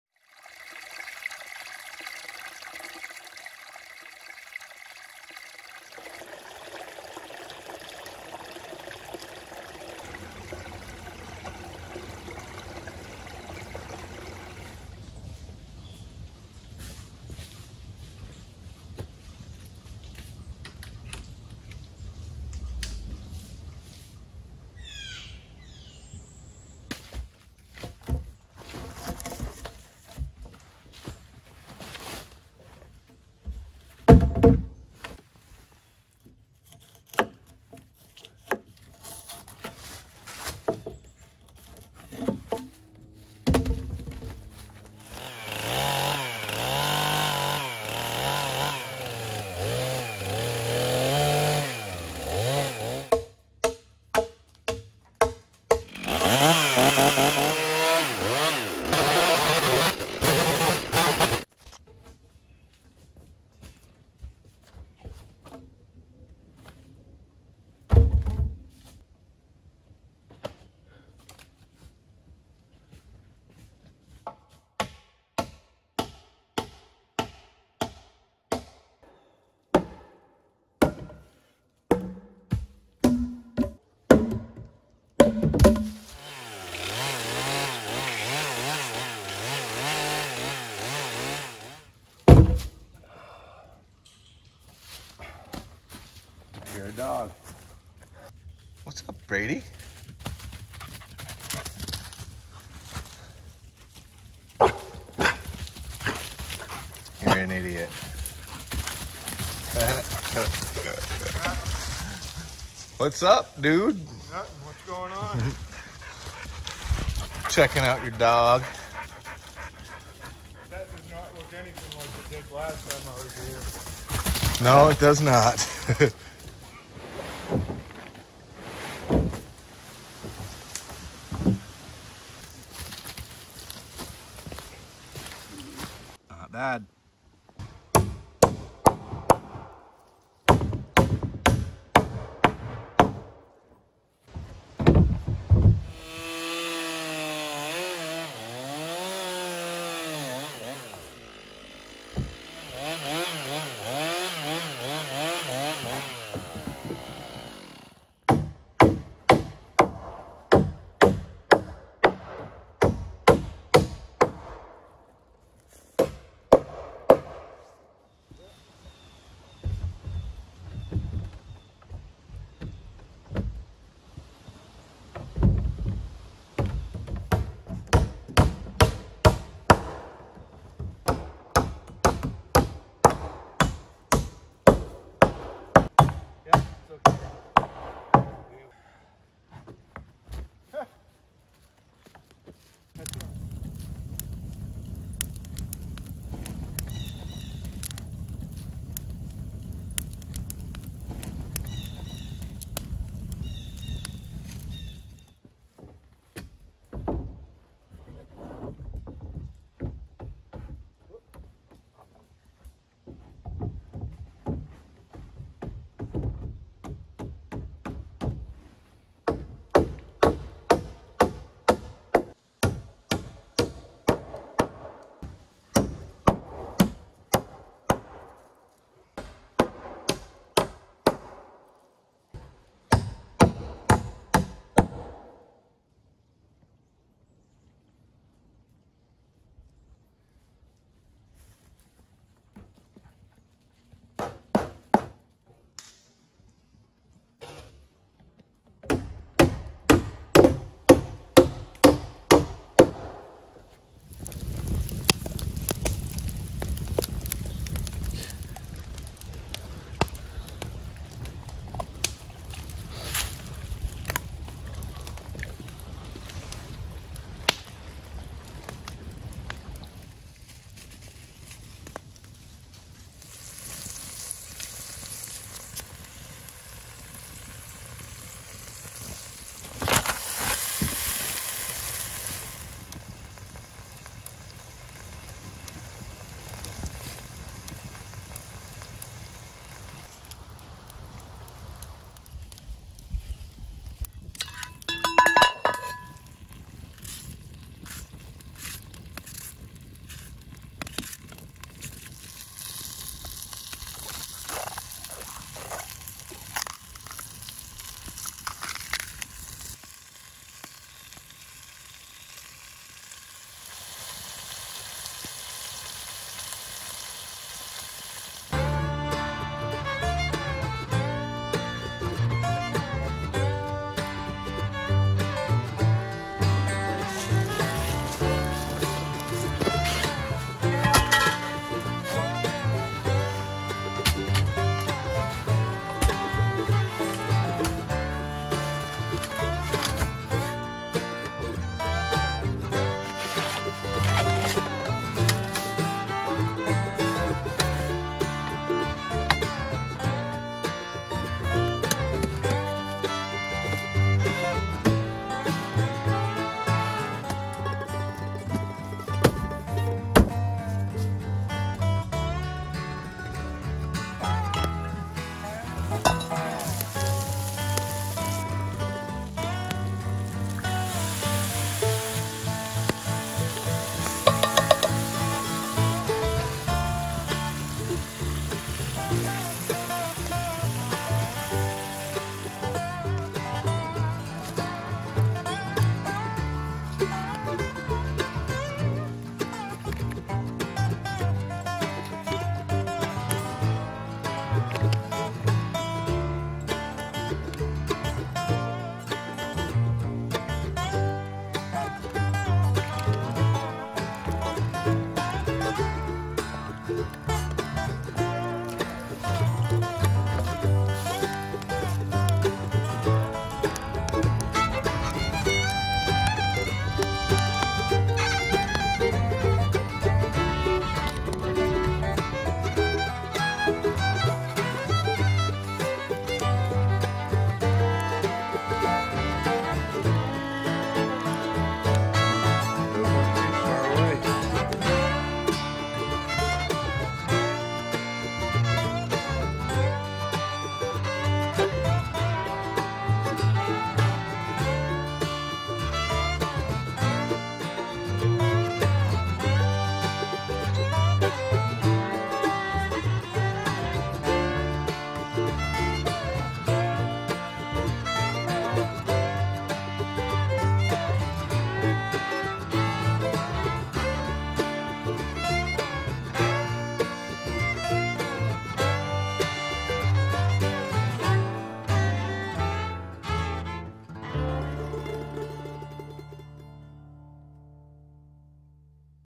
OVERNIGHT at THE LEAN TO! Starting the roof/CAMPFIRE Dutch Oven CHILI with homemade bacon & sausage
I love seeing the progress on the lean to. It is rewarding in so many ways.  I now have the first logs in place for the overhang, which really gives me an idea of what this is going to look like.My cousin Chris came over for the evening to hang out, and he  helped me get the roof started! An extra set of hands is nice.  Now The project is really starting to look like something! Dina , Hayden and the dogs joined us for a little while as well. Some campfire cooking to seal the deal. Chili in a dutch oven! Made with home cured bacon and homemade sausage from our pigs that we raised and processed.  Absolutely delicious for a cold night! Campfire cooking, Perfection. ! Does it get any better?!?!?!
   I spent the night under the structure out in the open, on my Eureka camping cot, with my Outdoor Vitals 0degree Summit sleeping bag, The temperature got down to 30 degrees and I was snug as a bug. It all made for a good time out in the woods. I hope you enjoy the video.